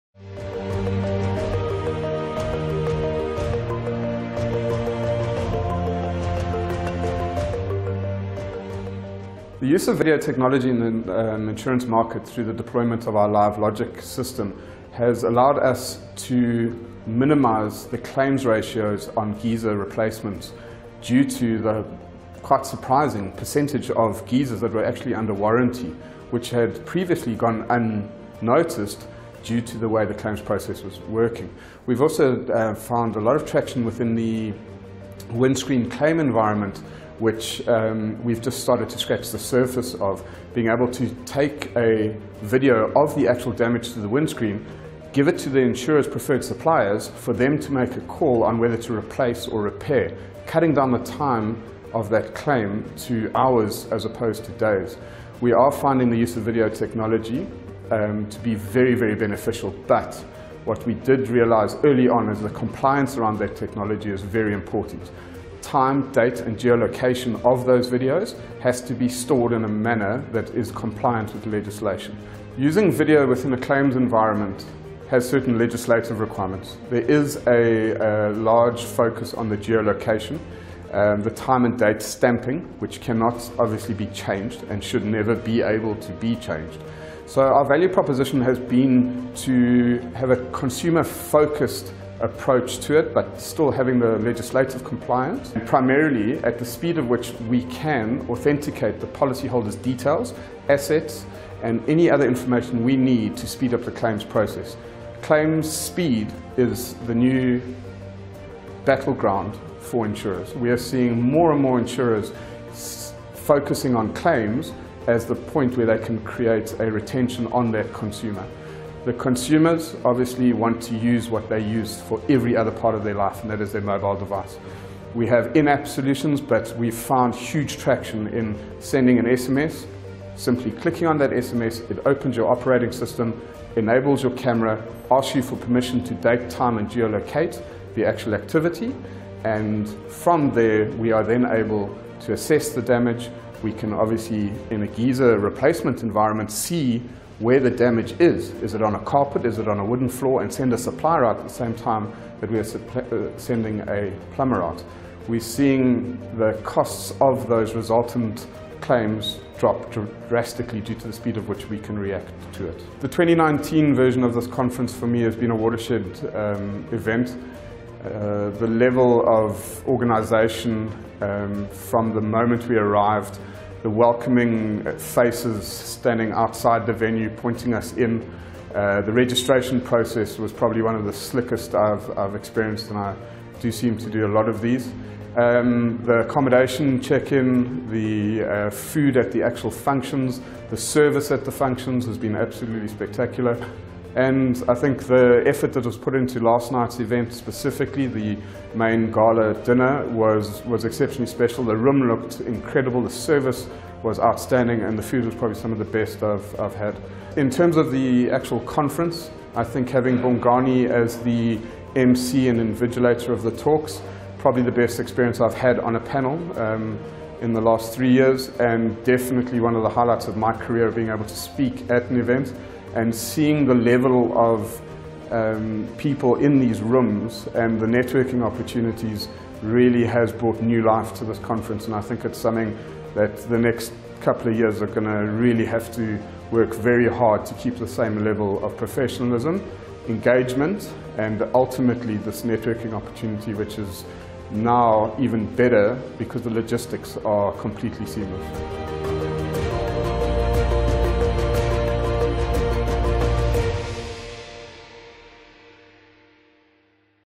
AIE interviews - Ronald Gall, Claim Central Africa...
Ronald Gall of Claim Central Africa sheds light on the use of video technology in the insurance environment. This video was shot at the 2019 African Insurance Exchange.